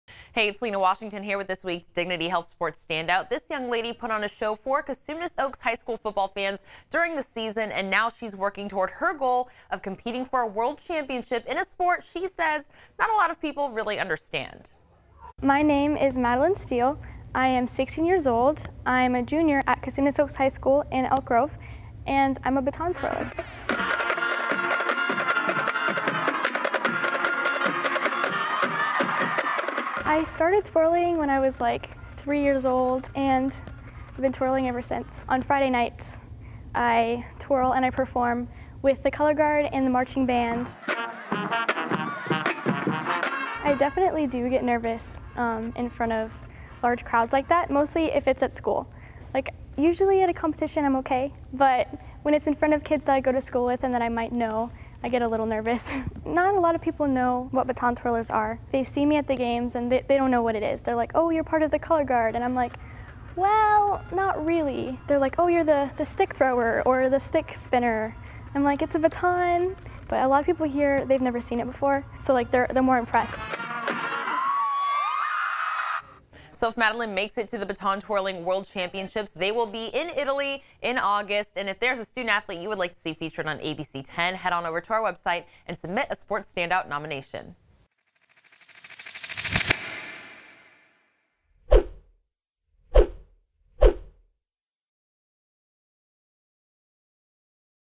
Sports Standout: Cosumnes Oaks High School feature baton twirler Madeline Stiehl
Cosumnes Oaks High School junior Madeline Stiehl explains how she first started baton twirling and what her competition goals are in this week's Sports Standout.